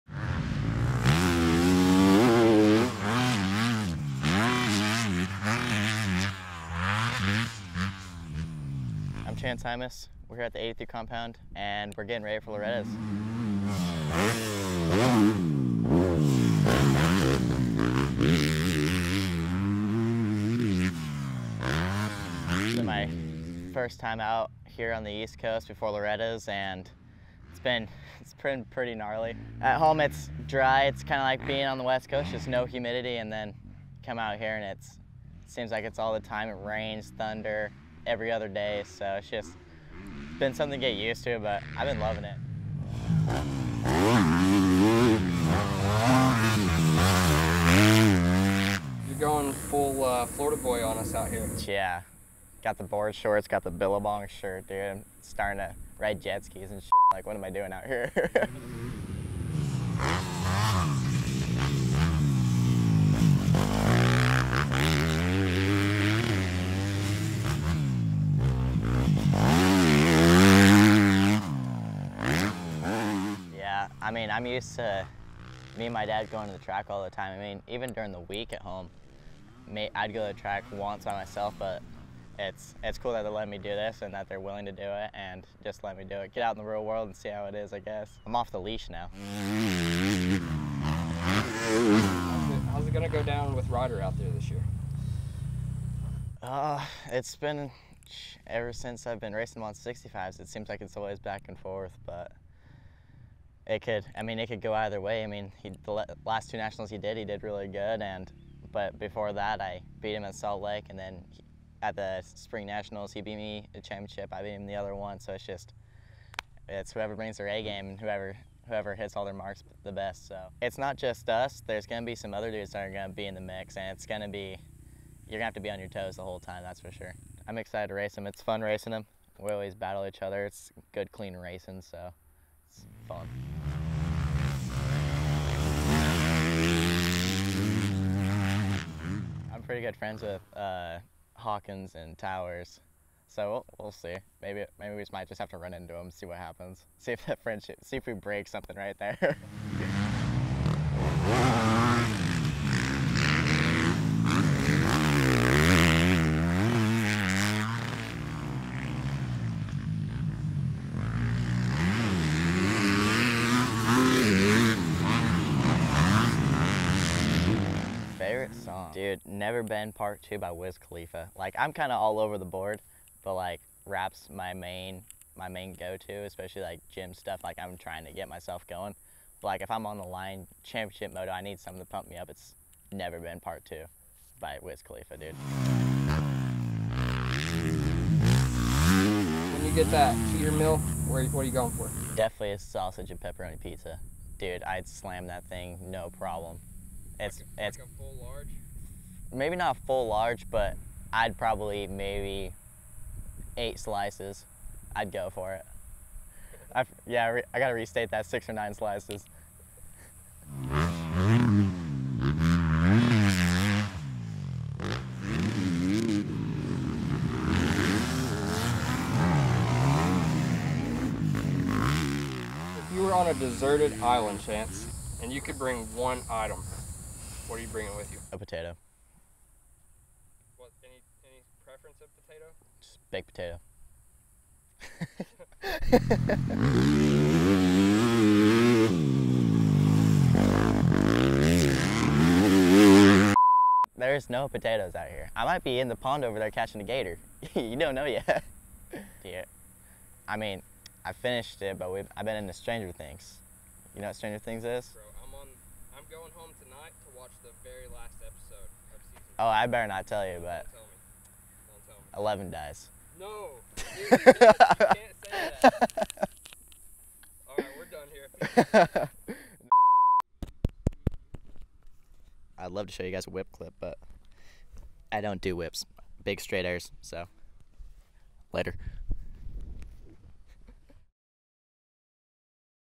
Florida Man Chance Hymas Preparing For Loretta Lynn's
Loretta Lynn's kicks off next week, so we stopped by 83 Compound in Florida to check in with Idaho native and now self described Florida Man Chance Hymas to see how he was preparing and of course ask him about potatoes and more.